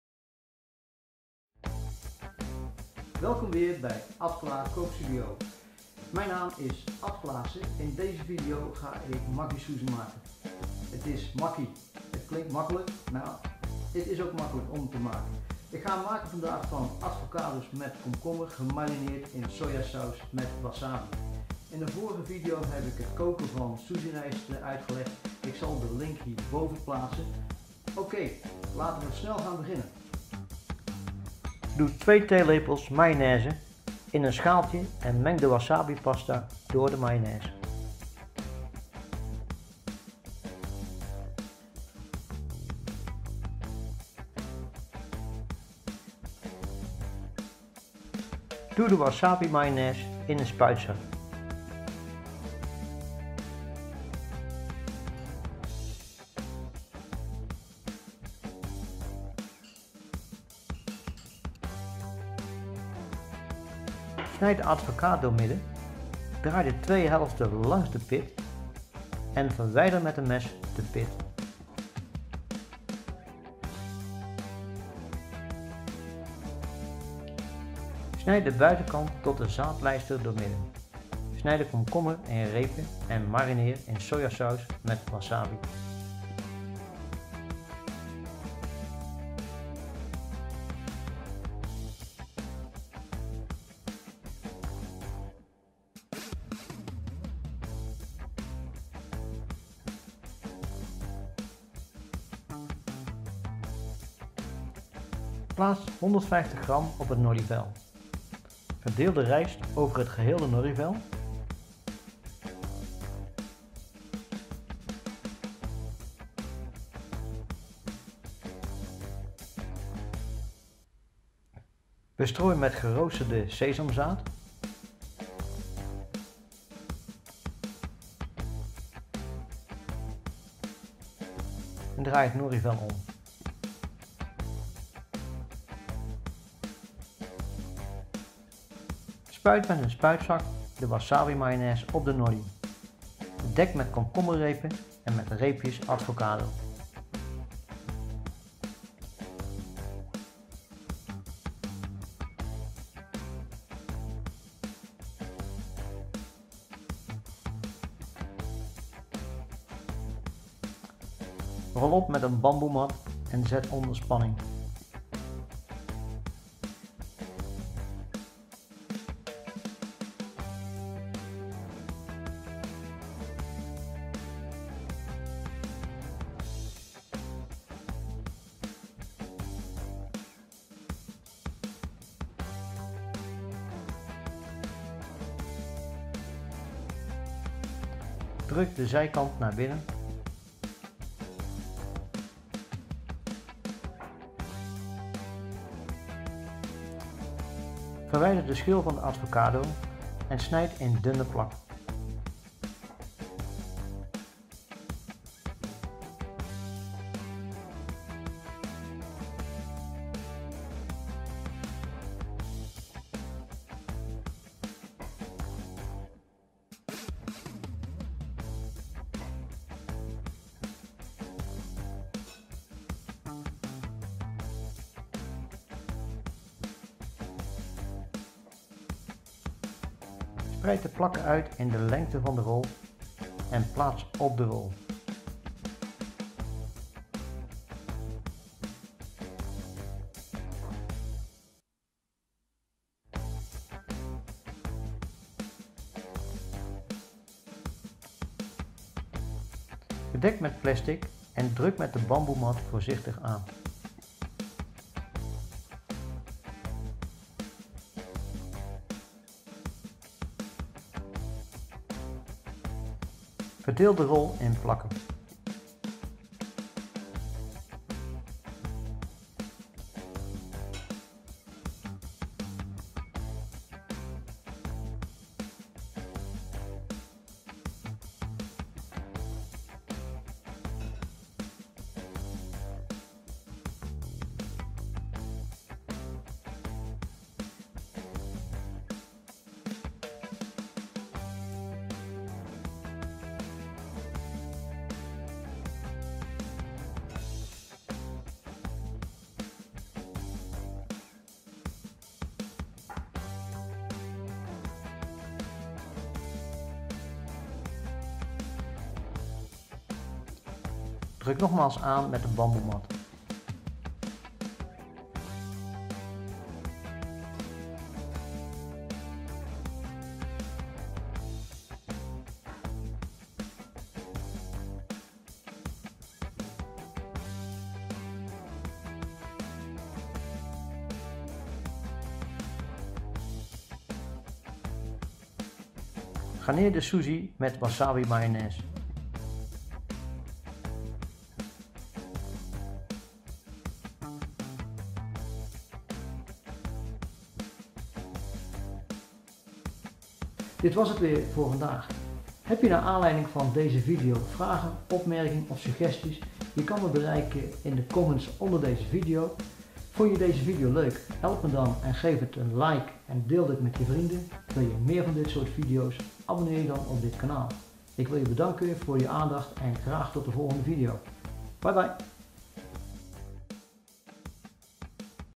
Sushi met avocado en komkommer
Sushi rol van avocado een combinatie van komkommer gemarineerd met sojasaus en wasabi, gerold in sushi rijst, geparfumeerd met geroosterd sesamzaad.

Hij is niet pikant dit komt doordat de wasabi gemengd is met mayonaise.

Is eenvoudig om te maken, kan als voorgerecht  maar ook als snack worden gebruikt.

Recept en omschrijving kunt u vinden door op de link hieronder te klikken.